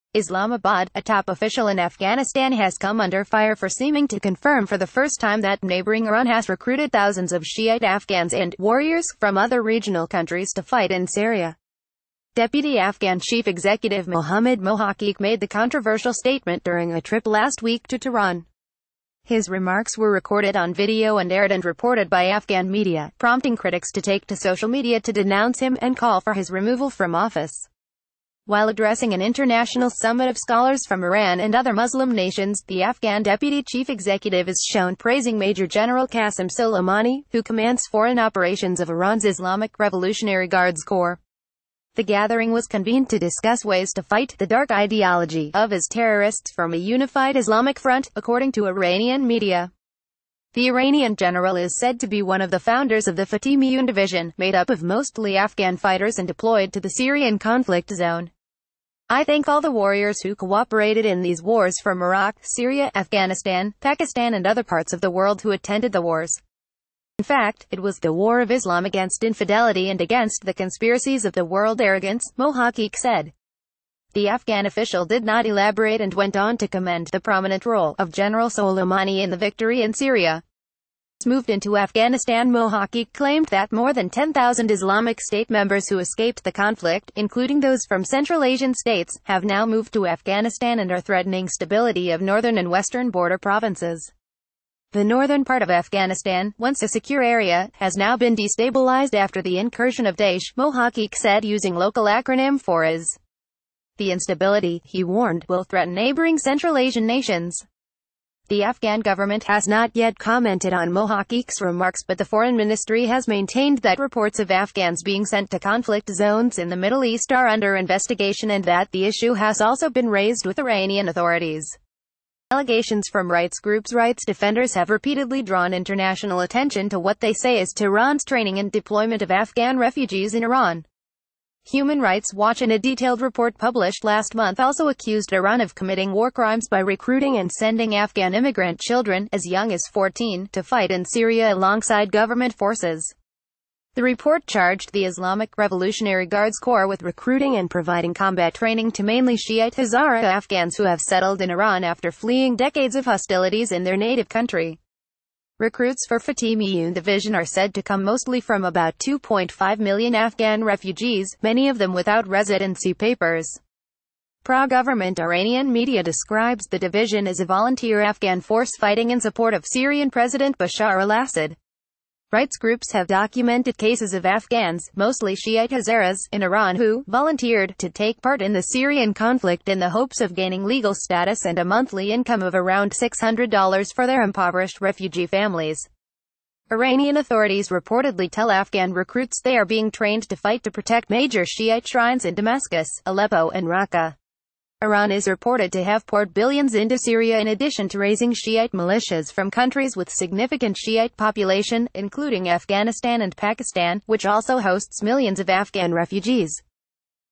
Us Latest News - Major thanks to iran for training in Afghanistan ' warriors ' for Syria ' victory '
Us Latest News - Us Latest News - Major thanks to iran for training in Afghanistan  warriors  for Syria  victory

ISLAMABAD —  A top official in Afghanistan has come under fire for seeming to confirm for the first time that neighboring Iran has recruited thousands of Shi’ite Afghans and “warriors” from other regional countries to fight in Syria. Deputy Afghan Chief Executive Mohammad Mohaqiq made the controversial statement during a trip last week to Tehran. His remarks were recorded on video and aired and reported by Afghan media, prompting critics to take to social media to denounce him and call for his r...